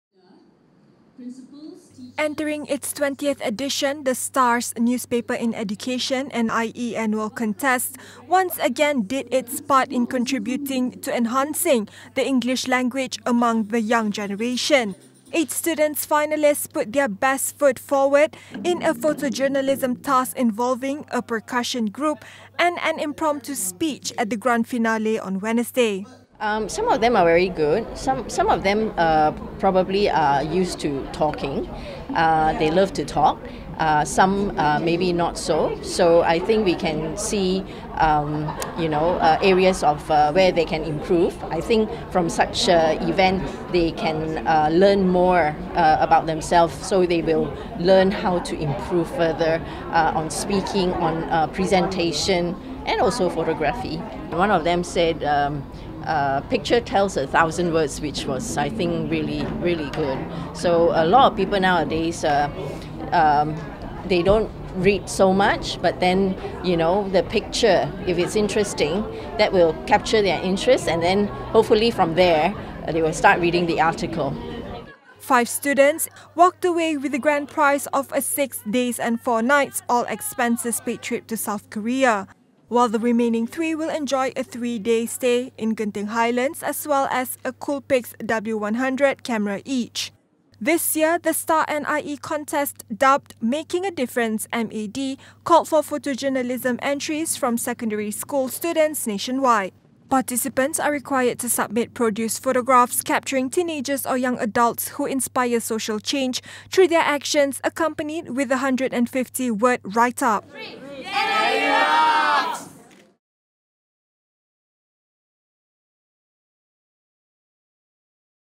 All expenses paid trip to S.Korea for NiE's winners
Eight students finalist who put their best foot forward in taking part in the 20th edition of The Star’s Newspaper-in-Education (NiE) annual contest were rewarded for their efforts at the grand finale on Wednesday.